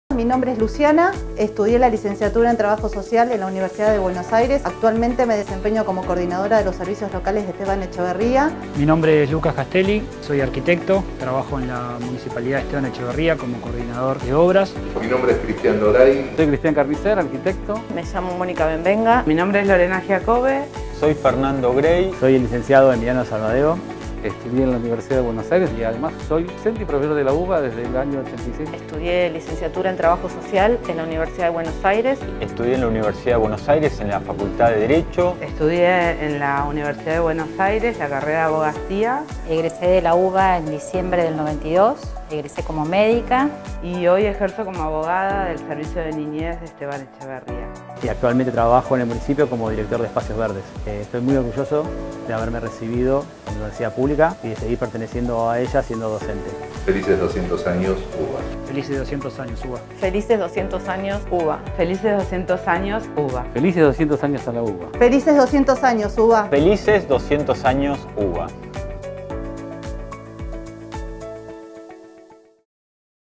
Fernando Gray - Felices 200 años a nuestra Universidad de Buenos Aires
Símbolo del acceso igualitario a la educación universitaria único en la región, emblema de la movilidad social ascendente, el gran orgullo académico y científico argentino, hoy conmemoramos el bicentenario de la querida Universidad de Buenos Aires. ¡Felices 200 años Universidad de Buenos Aires!